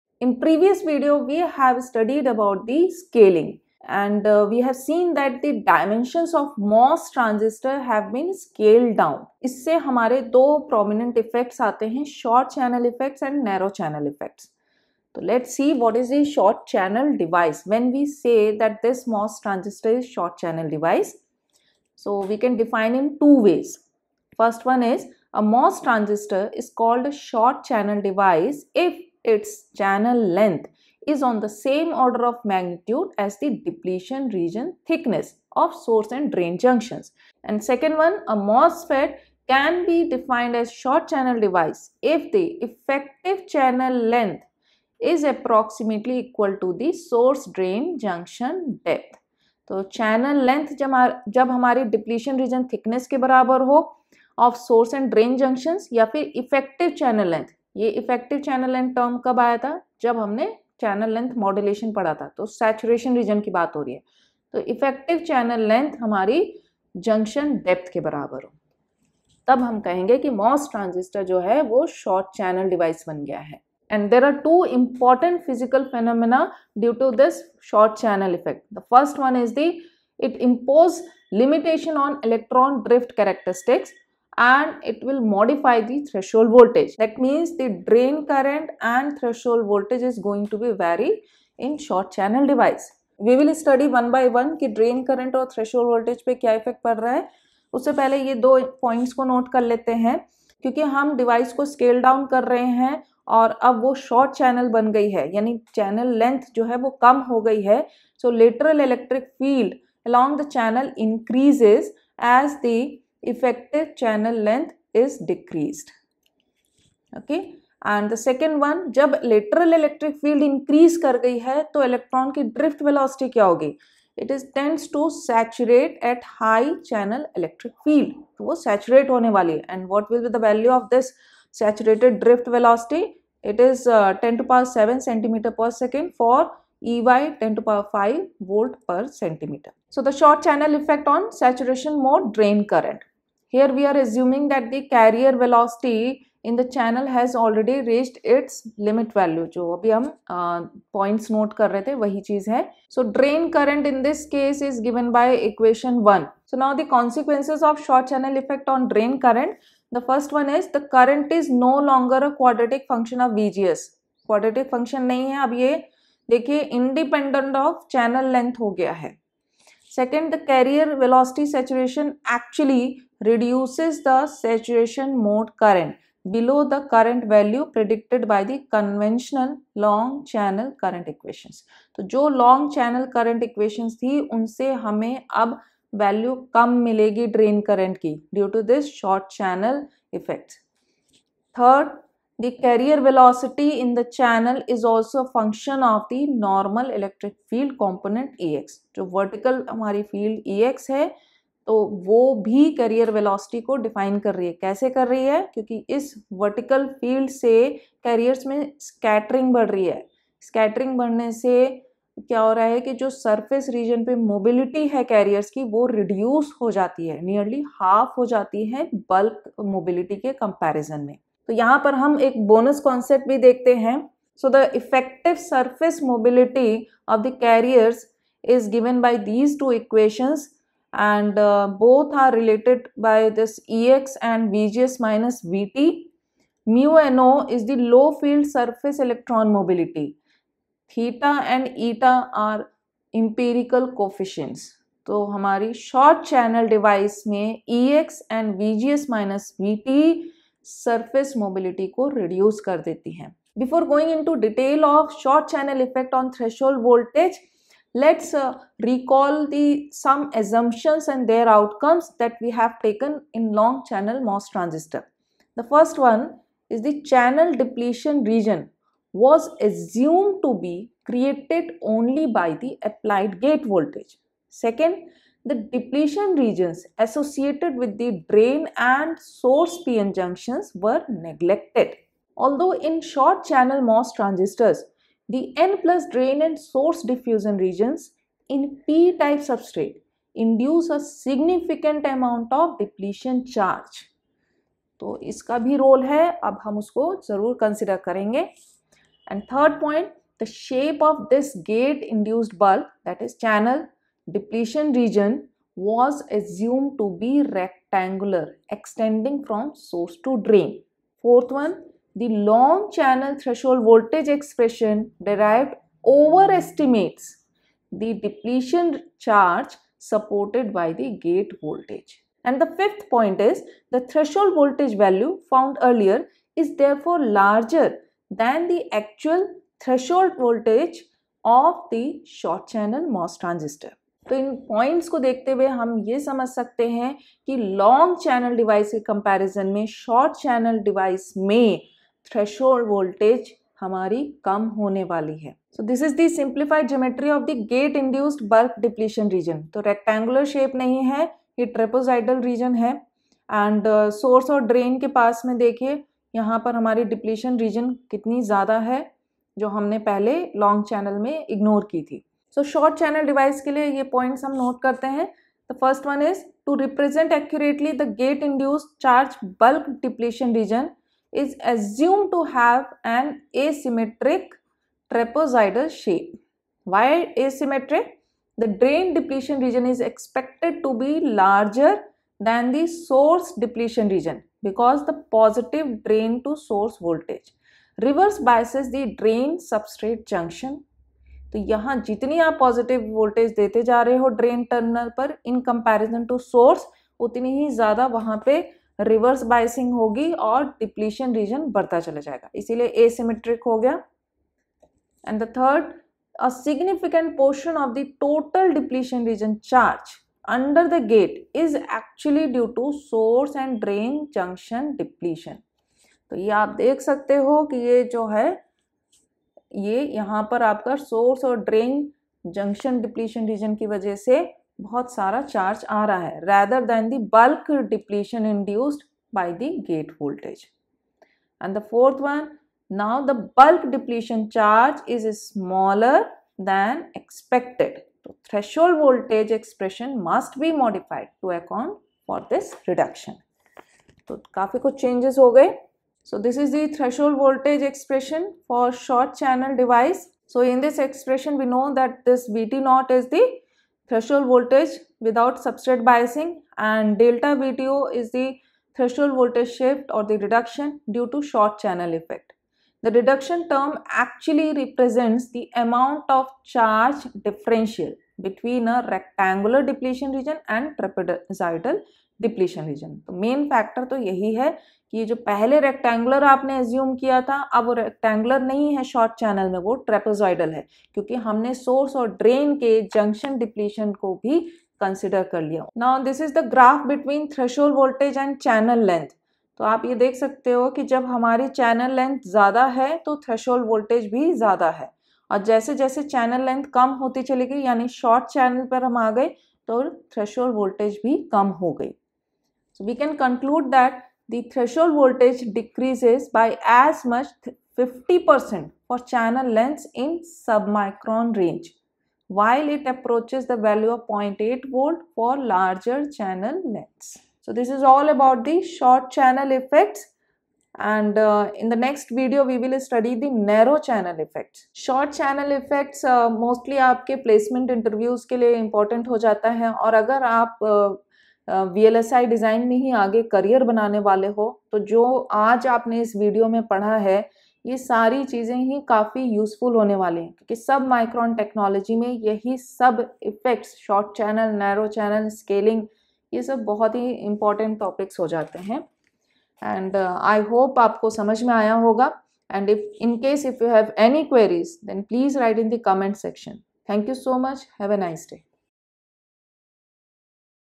short channel effects | MOSFET | vlsi Design
All about the short channel effects of a MOS transistor.

timestamps:
0:00 what is short channel device?
1:40 important points
2:20 effect on saturation current
4:15 effect on threshold voltage

Helpful to BTech, BE, MTech, MS, Research fellows and Faculties.